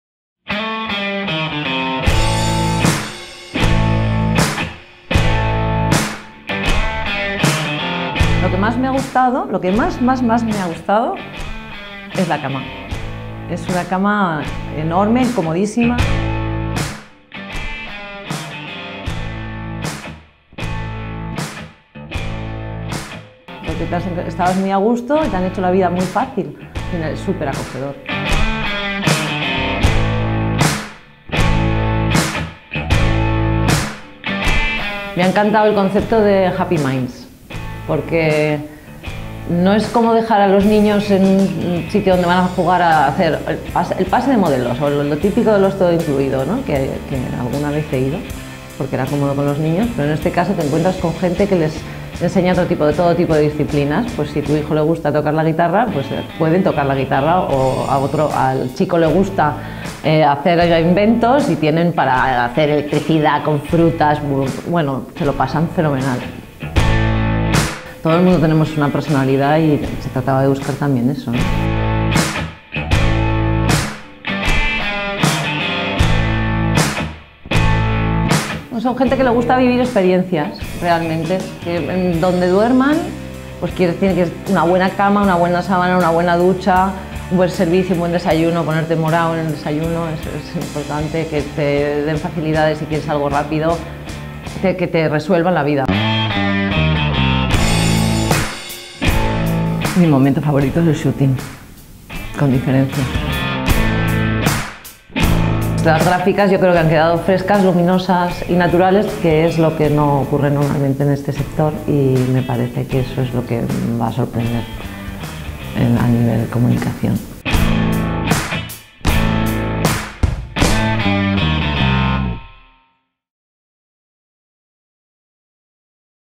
Occidental Hotels & Resorts
Making of de la campaña gráfica firmada por la fotógrafa Sara Zorraquino para Occidental Hotels & Resorts.